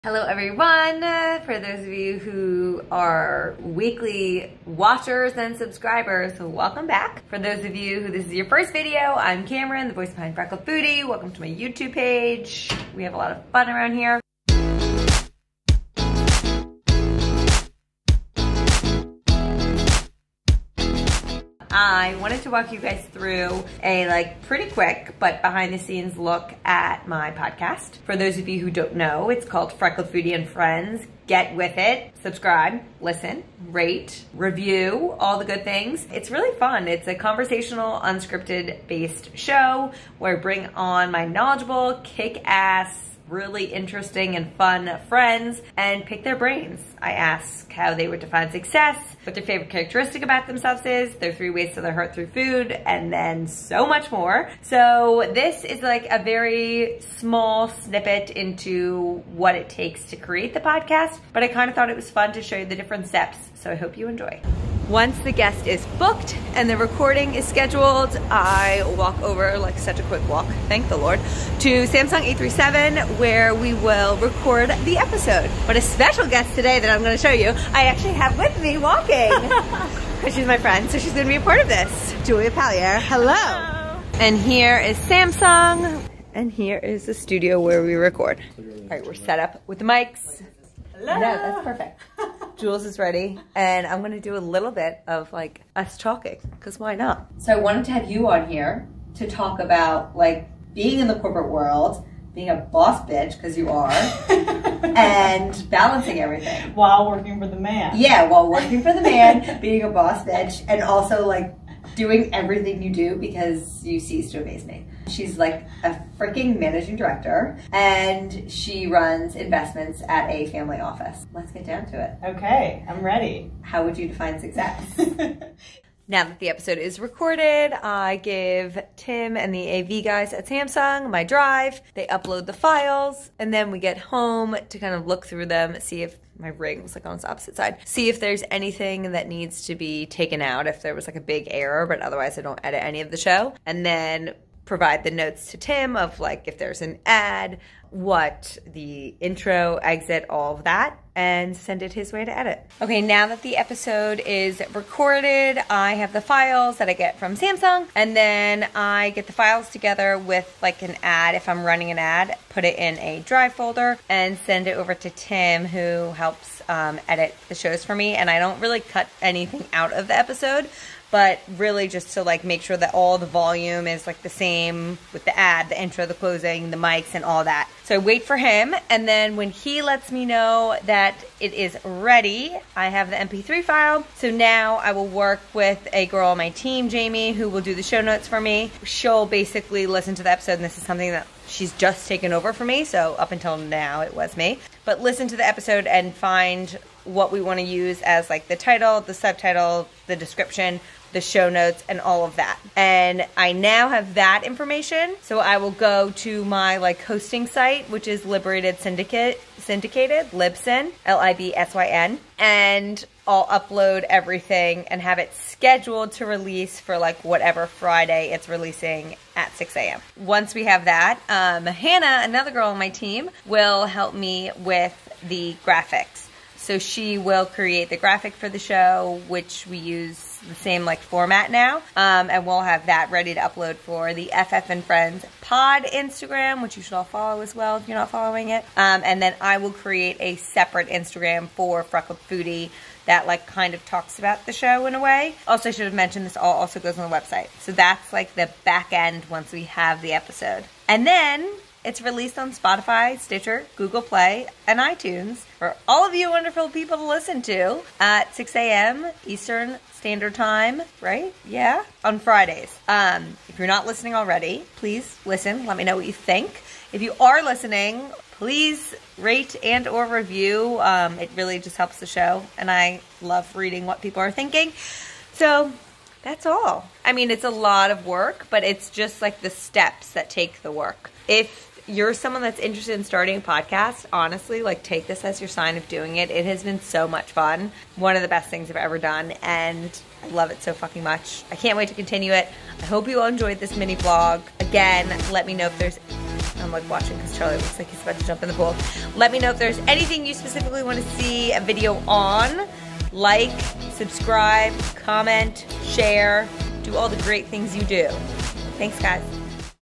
Behind the Scenes of the Podcast
Check out a behind the scenes look at what goes into recording my podcast, Freckled Foodie & Friends (link below). This VLOG shows you a look at the episode with one of my good friends, Julia Paliare, which releases tomorrow!